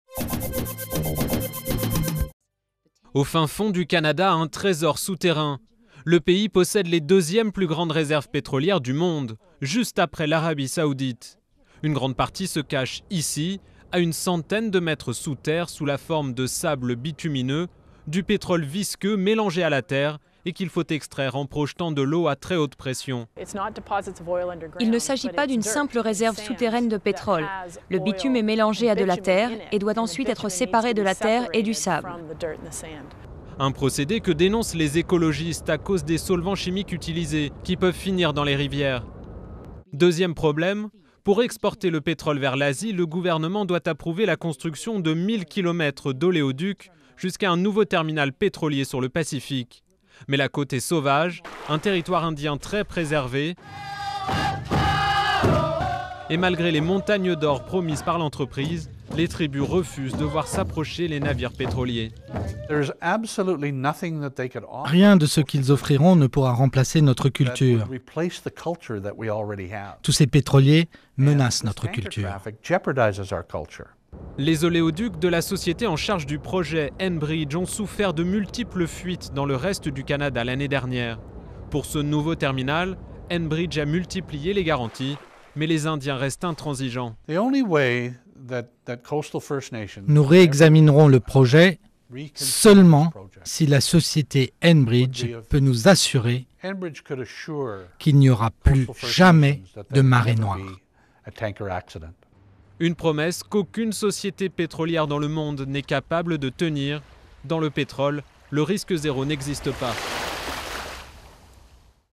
Le projet d'oléoduc canadien qui effraie les Amérindiens
Le projet est très controversé : plus de mille kilomètres d'oléoduc à travers la nature canadienne, pour permettre l'exportation d'un pétrole issu de sables bitumineux. Les tribus indiennes craignent une marée noire, comme le pays en a tant connu. Un refus obstiné malgré les incitations financières. Durée: 01:52